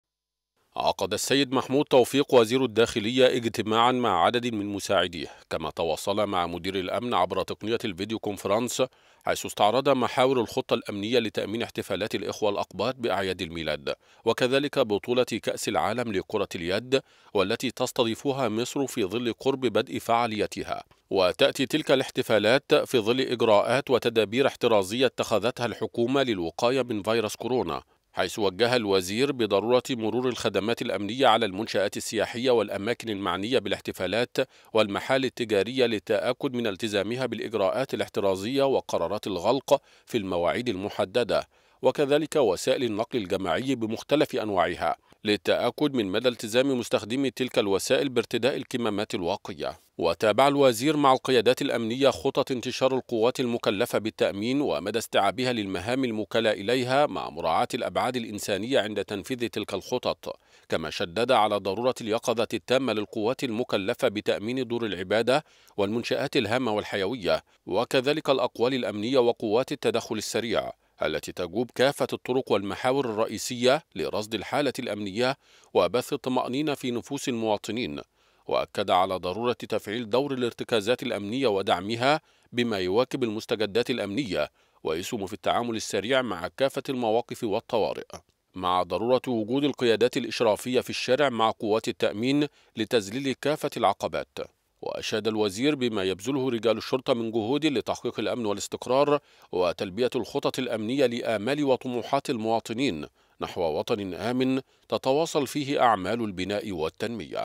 غرفة الأخبار| وزير الداخلية يجتمع مع عدد من القيادات الأمنية لبحث استراتيجية العمل الأمني
30 ديسمبر 2020